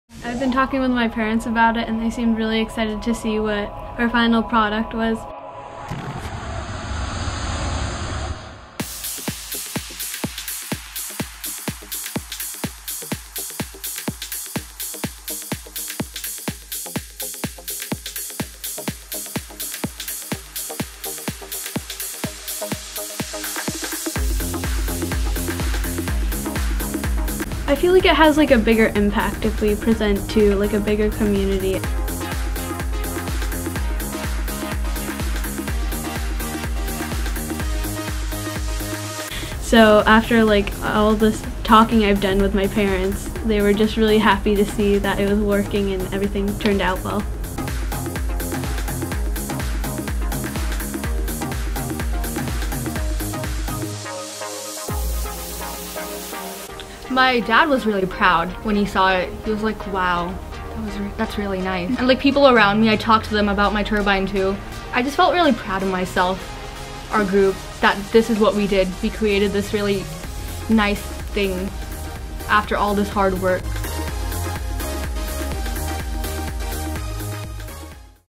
What makes school exhibitions so powerful?
At Manchester Elementary-Middle School, in Manchester VT, the whole school convened one evening for an exhibition of student work. And while everyone's been to a school exhibition before, hearing what exactly makes the experience so valuable for students may surprise you.